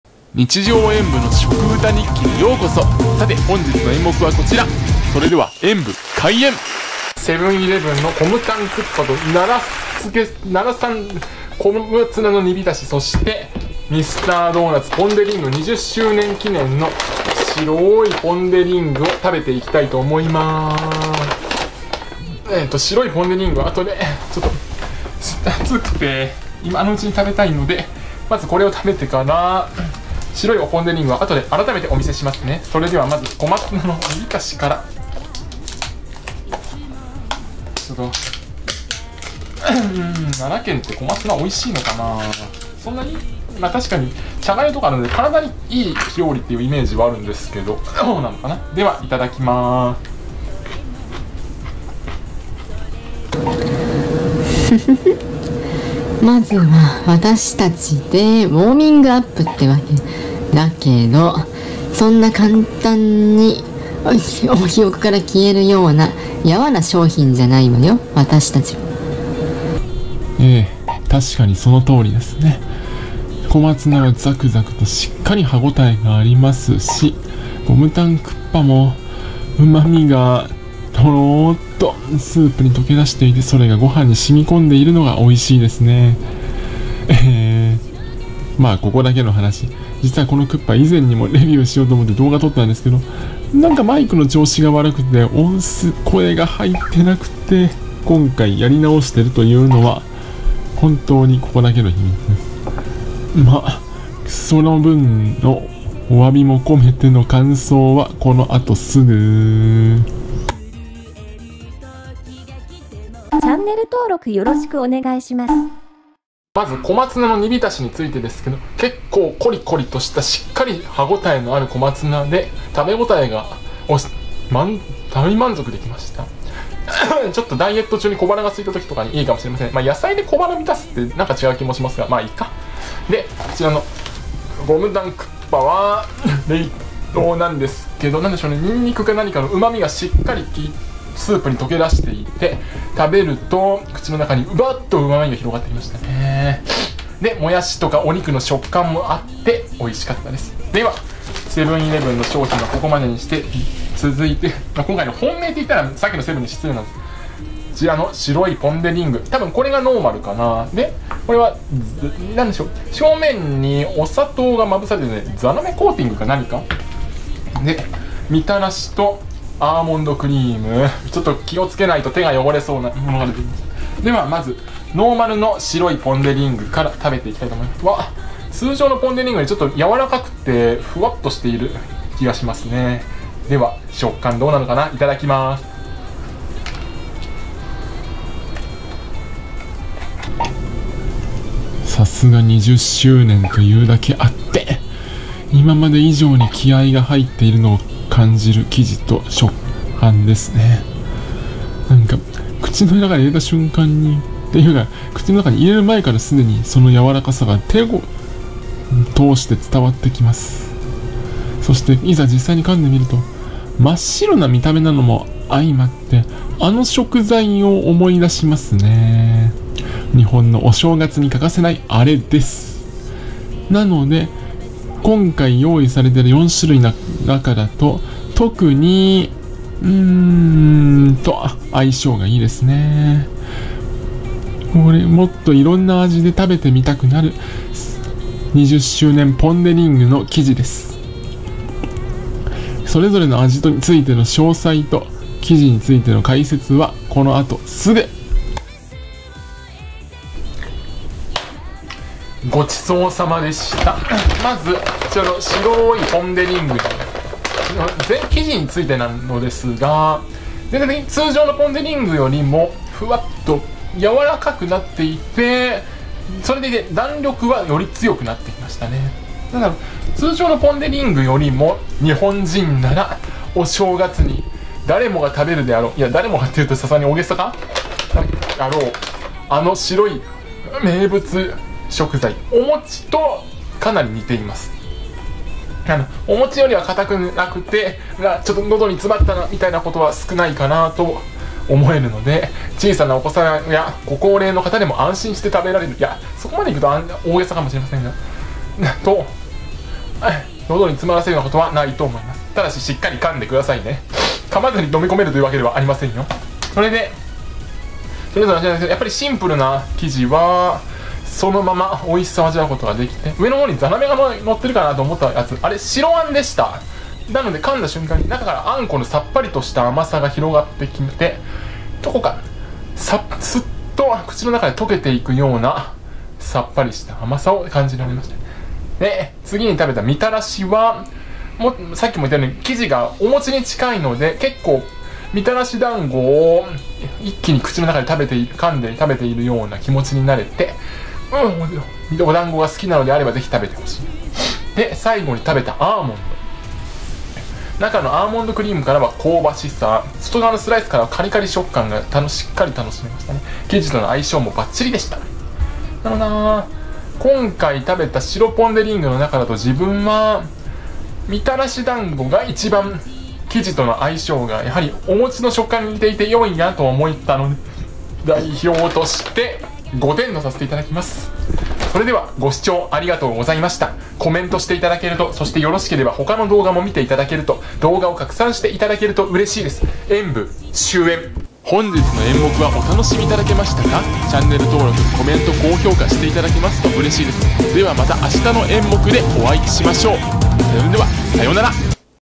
【VLOG】ミスドの「ポン・デ・リング」20周年記念商品を食べてみたら、〇〇に似ていました！！
「絵をかく前の画用紙みたい！！」
2023年に20年目を迎える、
ミスドの「ポン・デ・リング」記念商品
「白ポンデ4種」を私も食べてみました～
〇〇に似た食感と味、あなたも楽しみませんか？